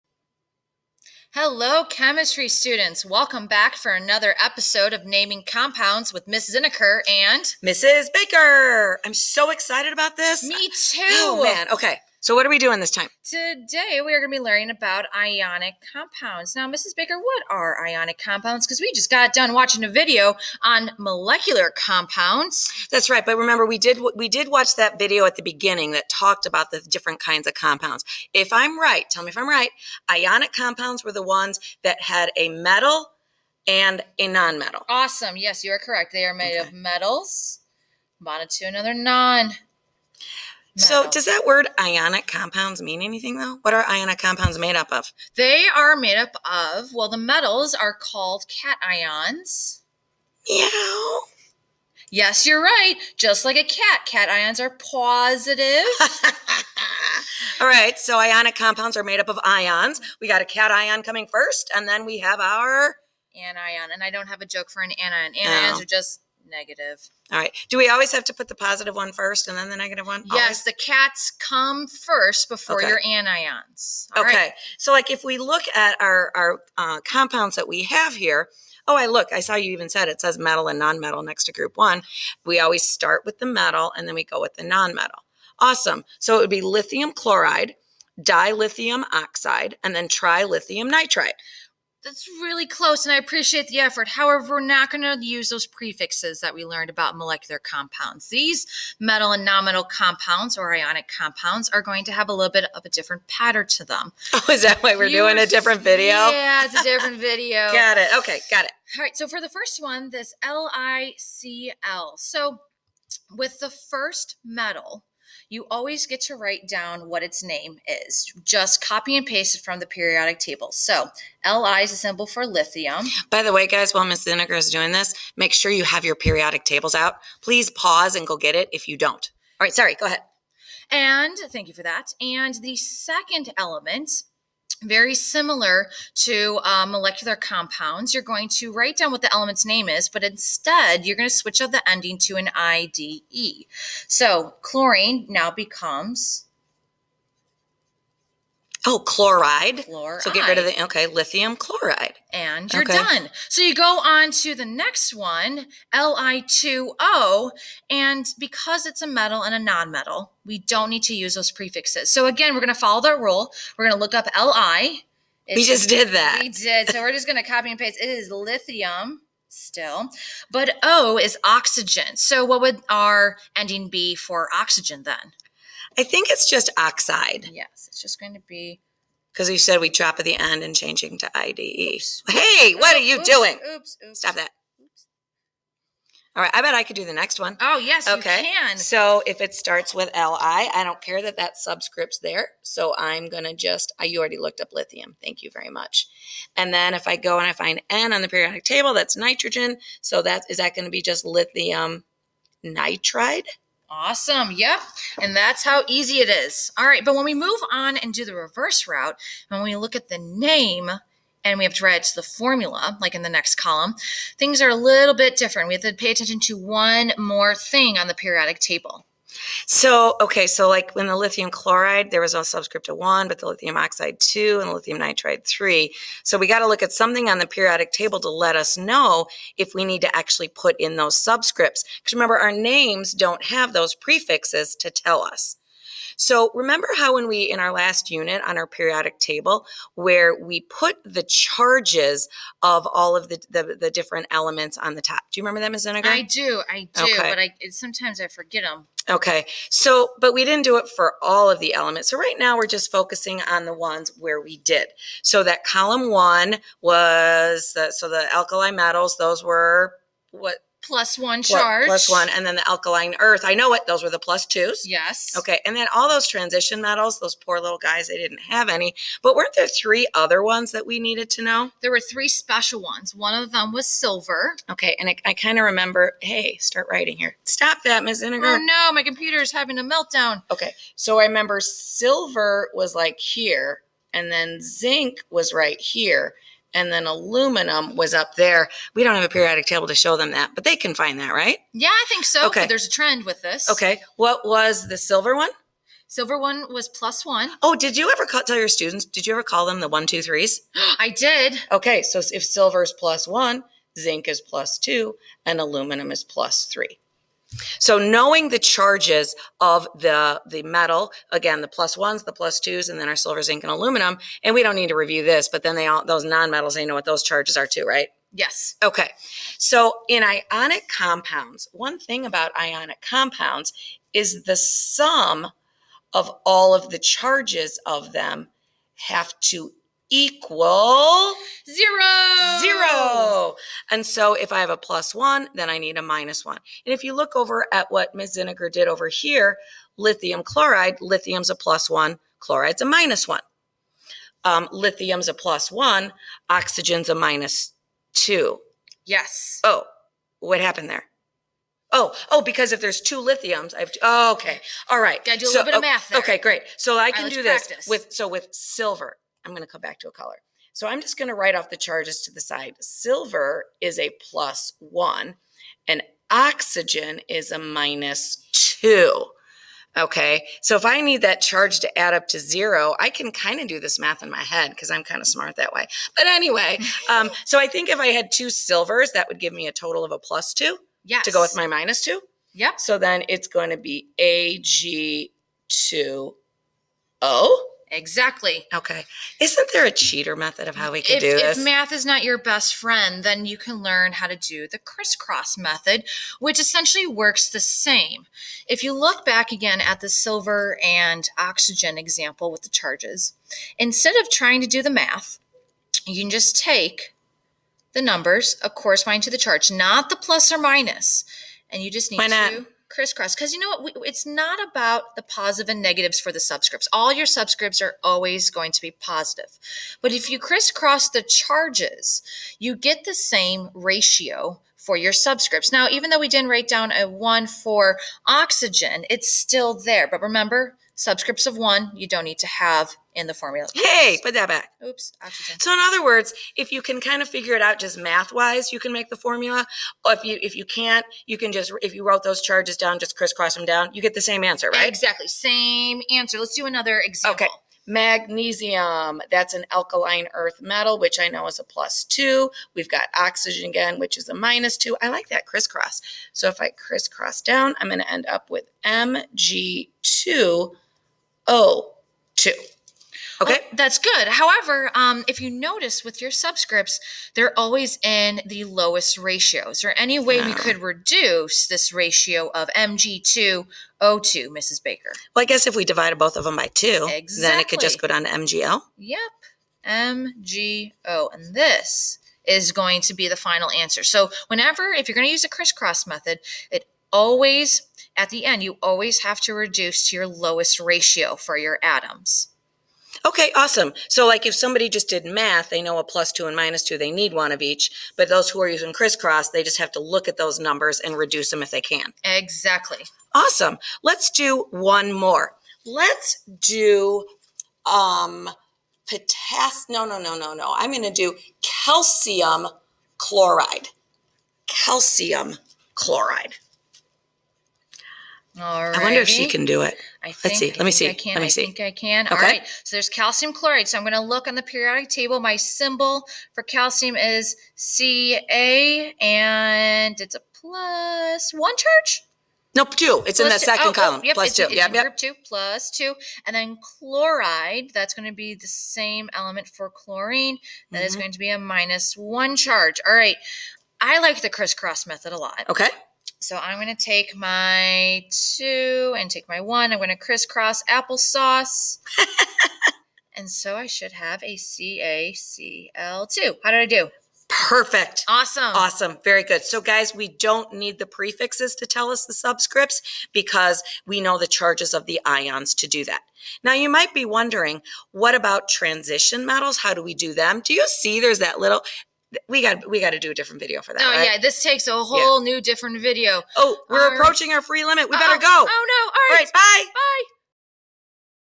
Video #3 Naming Ionic Compounds Video Notes
This video covers the naming notes for ionic compounds.